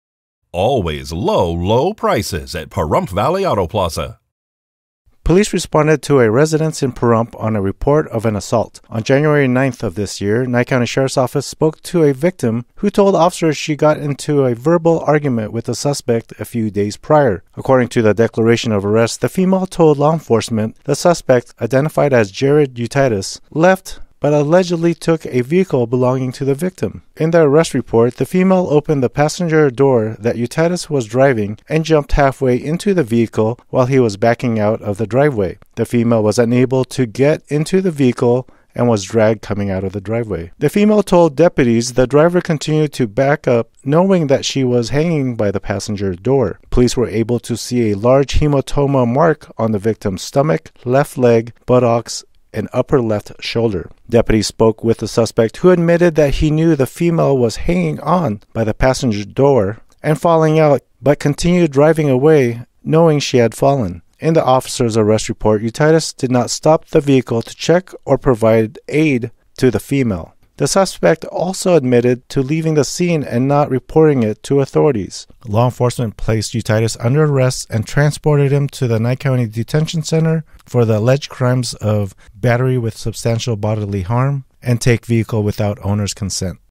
01/17/2023 Victim Injured During Vehicle Theft
A suspect is arrested for taking a car while the owner hangs on to the passenger door.